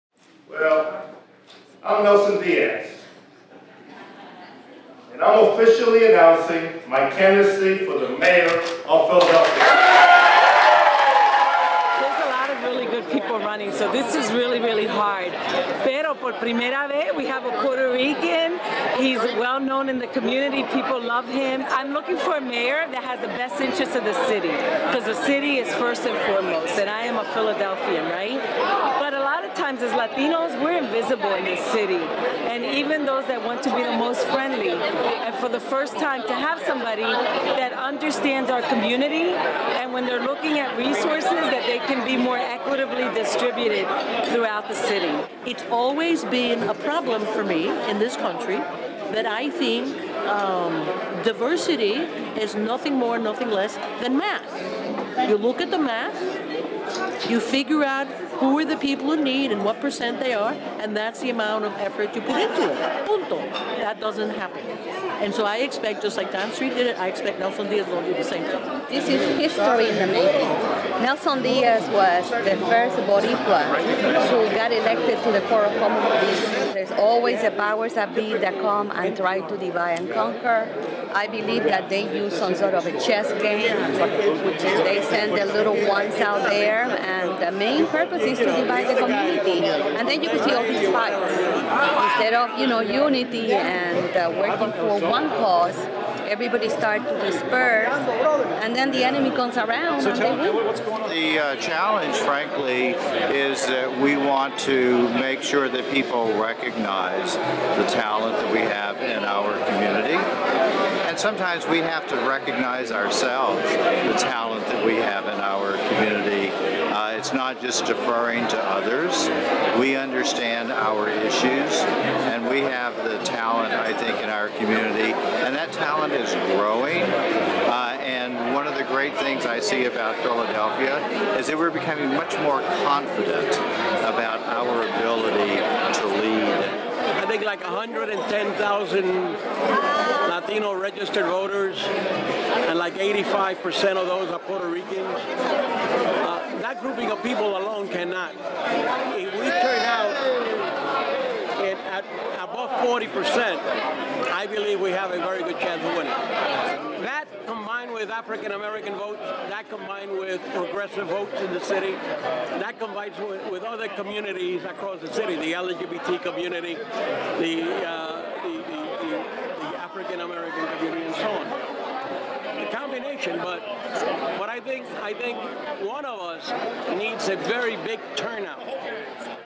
Nelson Diaz: Philadelphia's first Puerto Rican mayoral candidate
Latino leaders and activists from around the city comment on what a Puerto Rican candidate for mayor means for Philadelphia, and the chances of the city electing a Latino mayor this year.